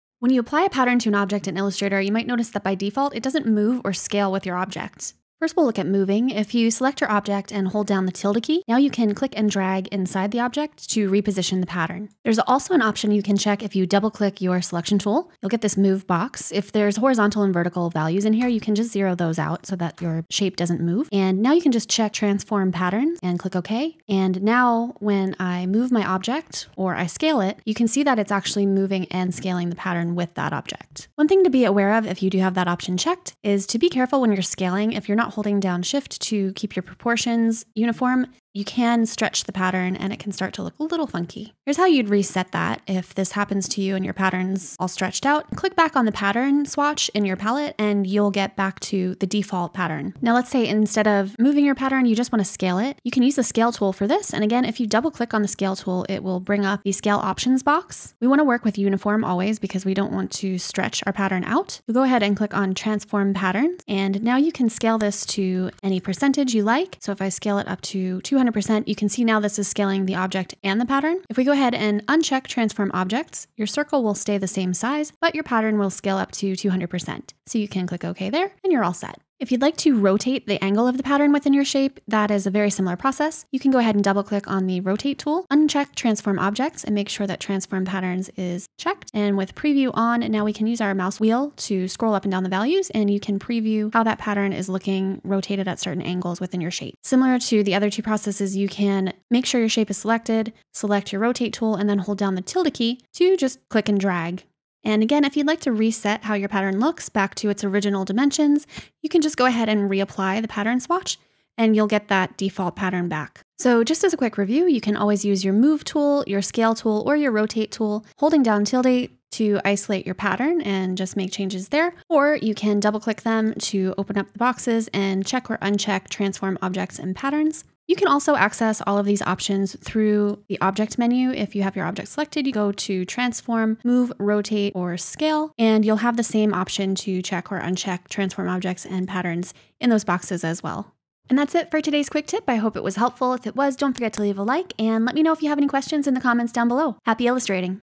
How to Move, Scale, Rotate or Reset a Pattern in Adobe Illustrator
By default, patterns don't scale or move with objects in Adobe Illustrator. Learn how to scale patterns, move them and even rotate them within your shapes using the tilde shortcut key (~) or by accessing the dialog boxes within the move, scale, and rotate tools.

00:00 - Intro
00:05 - Moving a Pattern Within a Shape
00:38 - Resetting a Pattern
00:51 - Scaling a Pattern Inside a Shape
01:18 - Rotating a Pattern in Adobe Illustrator
01:52 - Review, Moving, Scaling & Rotating Patterns

Cheers!
Kat